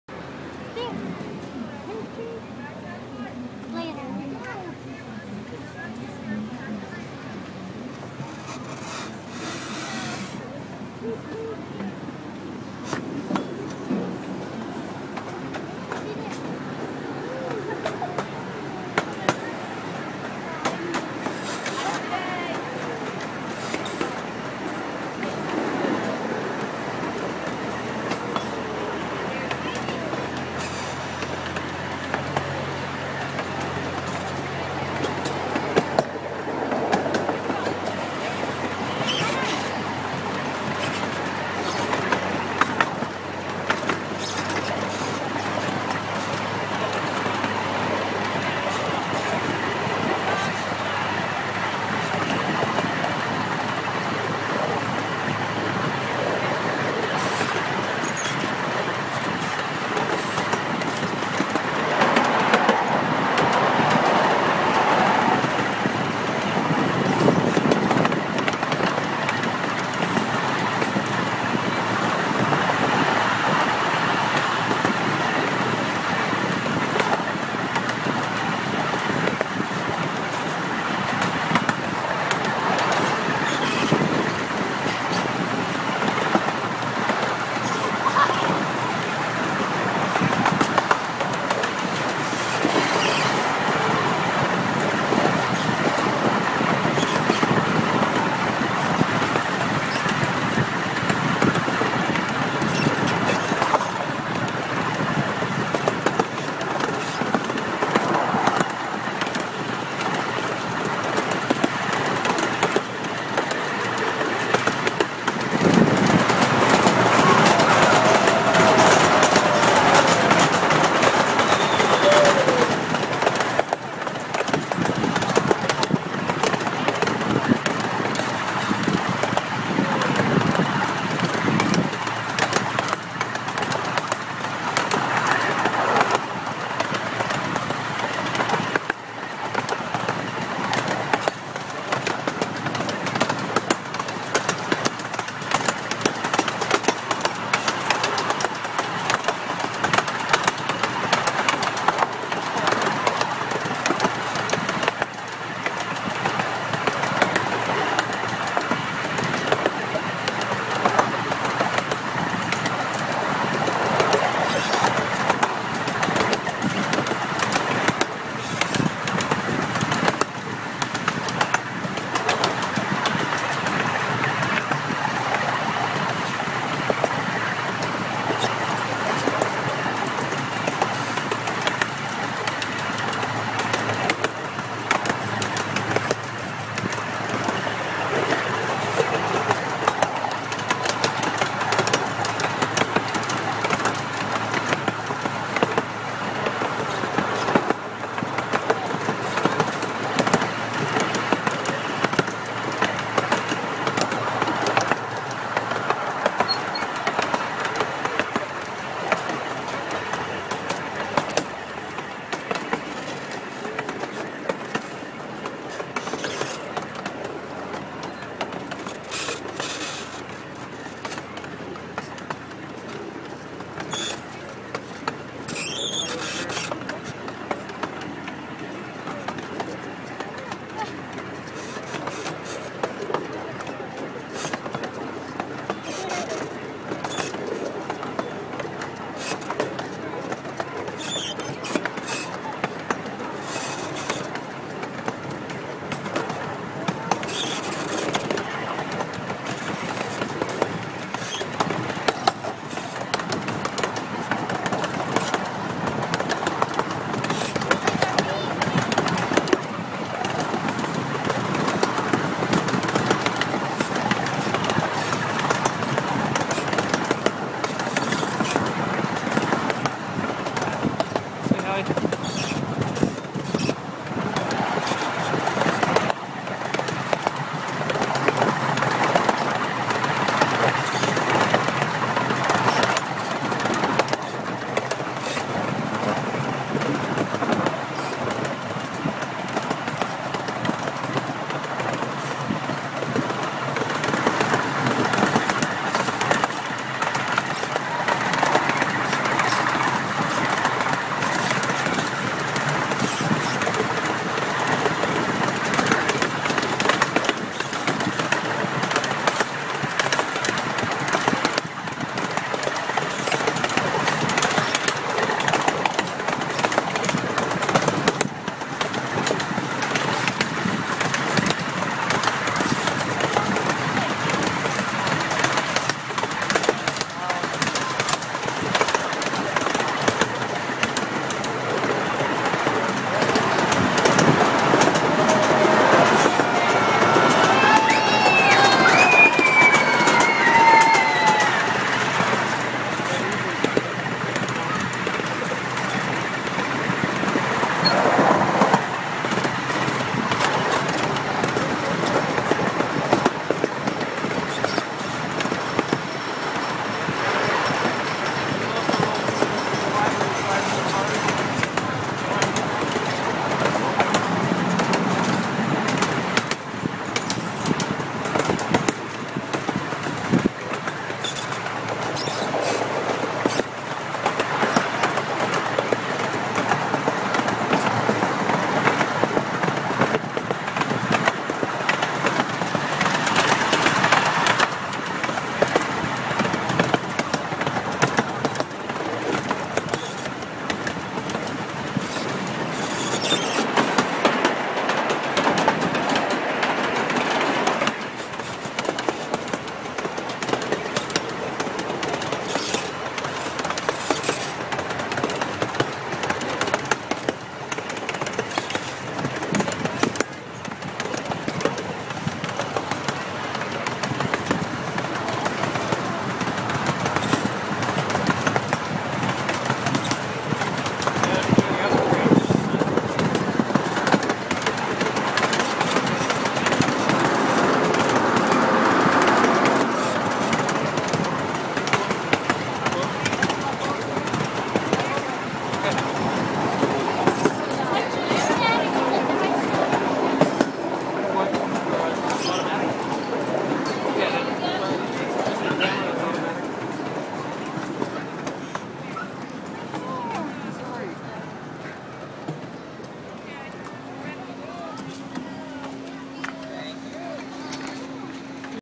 Knoebels Grove Train
Knoebels Grove Train ride, view from right side of car using my new iPhone 4. Video from 2 September 2010
Sorry about the sun glare, no hood on the iPhone :P